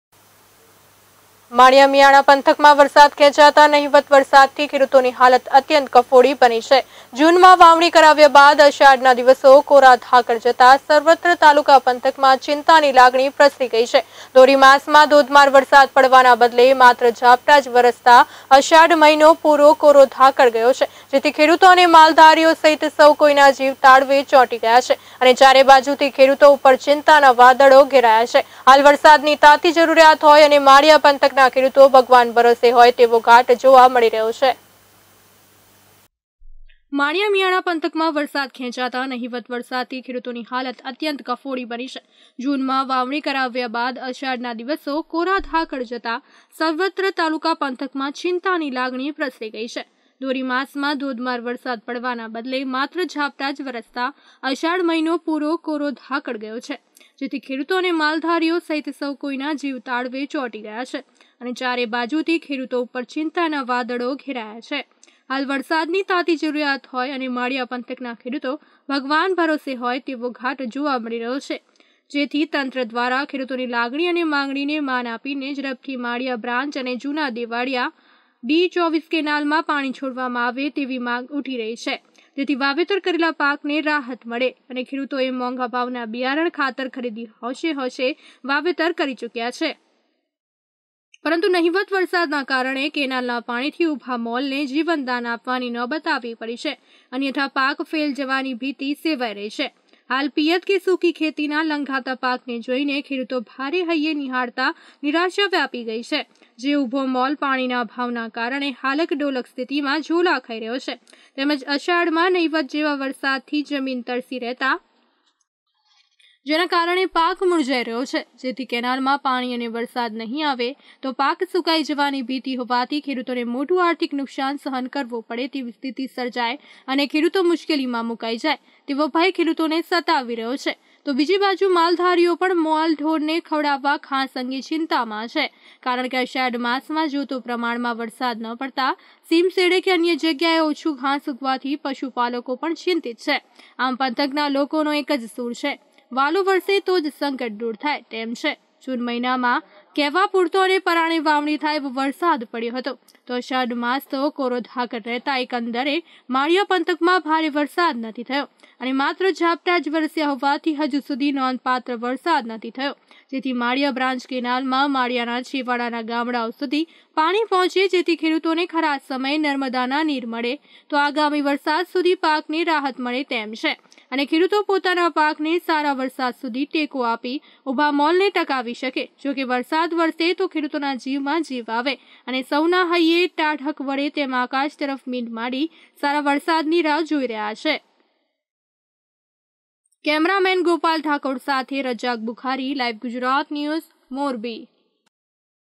મોરબી : માળીયામિંયાણા પંથકમાં ધોરી ગણાતા અષાઢ માસમાં માત્ર ઝાપટા વરસ્યા જગતાત ચિંતિત પંથક કોરોધાકડ
માળીયામિંયાણા પંથકમાં વરસાદ ખેંચાતા નહીવત વરસાદથી ખેડુતોની હાલત અત્યંત કફોડી બની છે જુનમાં વાવણી કરાવ્યા બાદ અષાઢના દિવસો કોરાધાકડ જતા સર્વત્ર તાલુકા પંથકમાં ચિંતાની લાગણી પ્રસરી ગઈ છે ધોરી માસમાં ધોધમાર વરસાદ પડવાના બદલે માત્ર ઝાપટા જ વરસતા અષાઢ મહીનો પુરો કોરોધાકડ ગયો છે. જેથી ખેડુતો અને માલધારીઓ સહિત સૌ કોઈના જીવ તાળવે ચોટી ગયા છે અને ચારે બાજુથી ખેડૂતો ઉપર ચિંતાના વાદળો ઘેરાયા છે હાલ વરસાદની તાતી જરૂરિયાત હોય માળીયા પંથકના ખેડુતો ભગવાન ભરોસે હોય તેવો ઘાટ જોવા મળી રહ્યો છે.